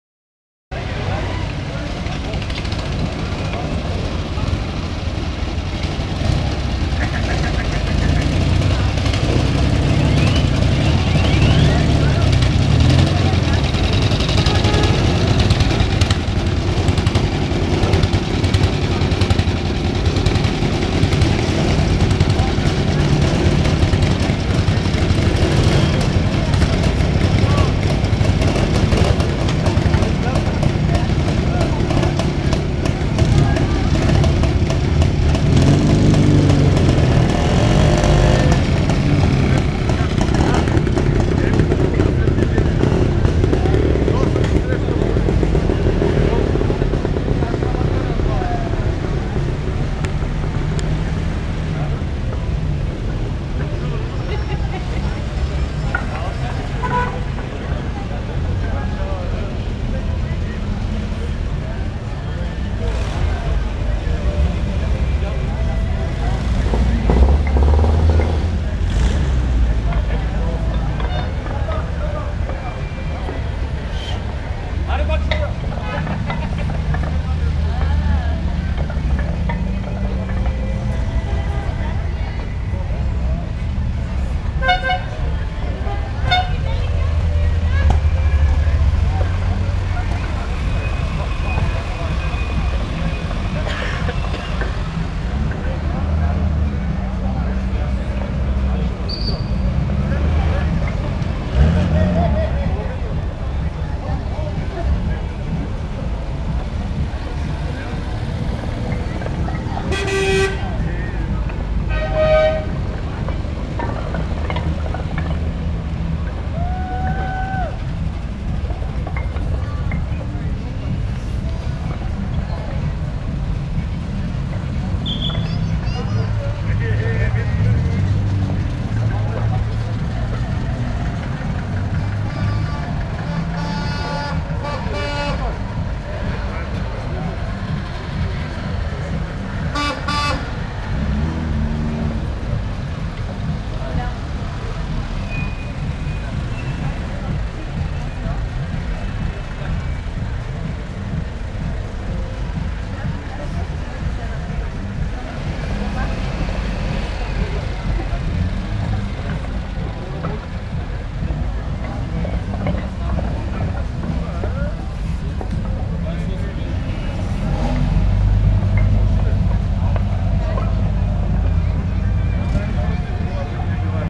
Ретро Рали Родопи
Стартира ретро ралито от Пловдив до Велинград